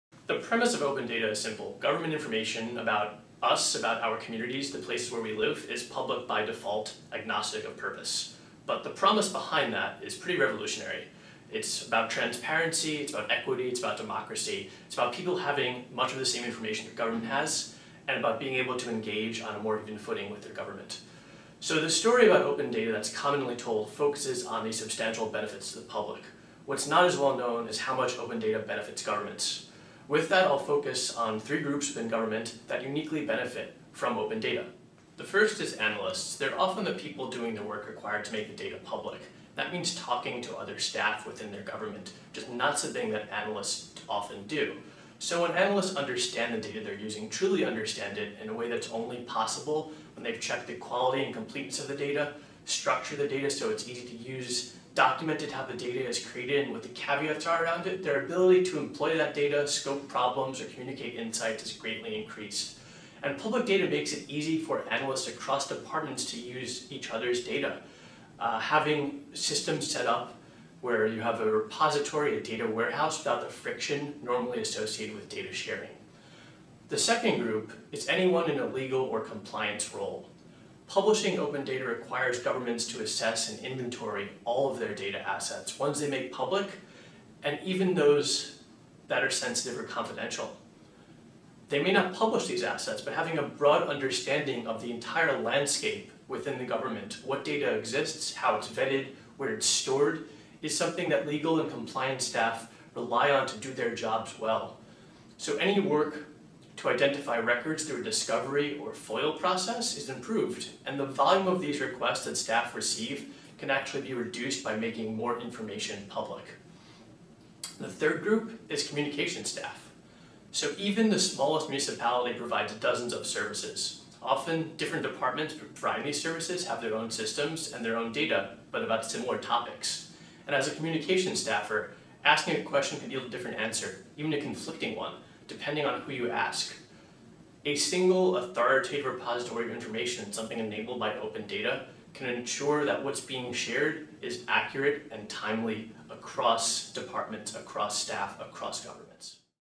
The Benefits of Using Open Data: Zachary Feder
Zachary Feder, Open Data Program Manager, NYC Mayor's Office of Data Analytics